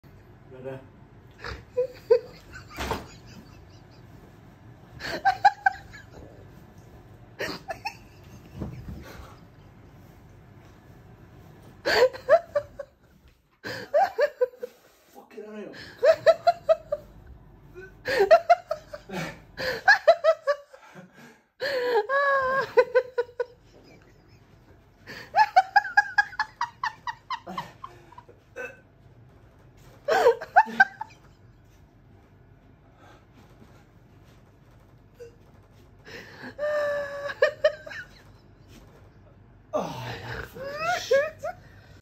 Boyfriend and Cat Working Together to Catch a Moth Fail || ViralHog
Occurred on June 9, 2022 / Leeds

My boyfriend lifted my cat Scarlett up one night after work to help her get a moth she couldn’t reach.